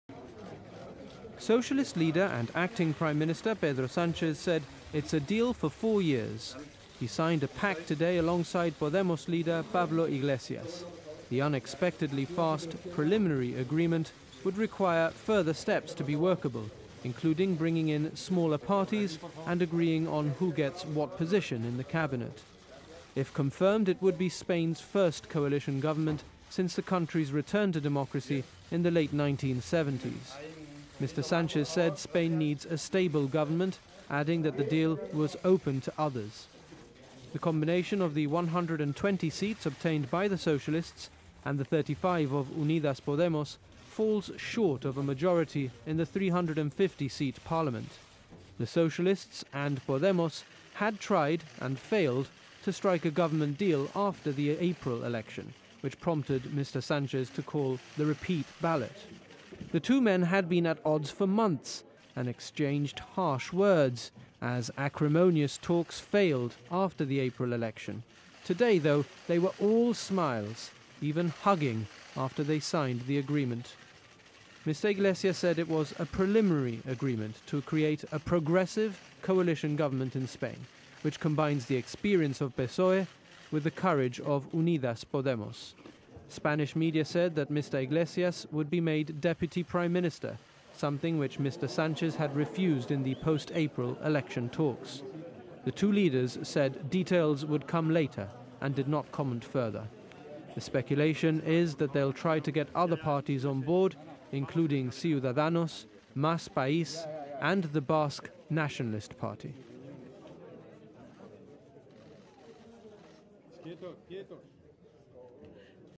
Spain's Socialists & far-left party Unidas Podemos agree on basis of a coalition government
Just two days after a parliamentary election delivered a highly-fragmented Spanish parliament, the Socialists and far-left party Unidas Podemos have agreed on the basis of a coalition government today.

The fourth election in four years left Spain's parliament even more divided than the previous ballot in April, with the PSOE retaining its lead but further away from a majority.